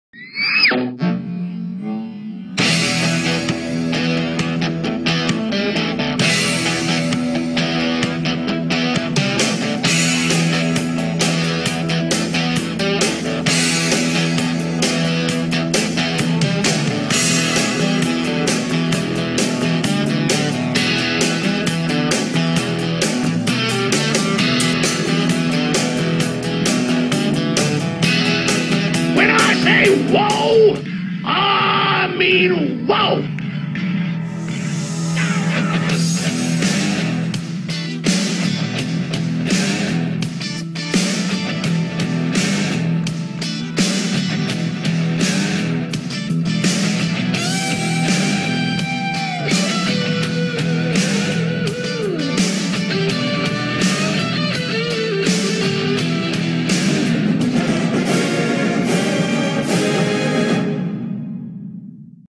My weekly Iowa Football game posts 2016
I create these for my Facebook friends, it's basically a 1 min video with special thanks to Dire Straits & Bad Company for a great soundtrack music to mix together.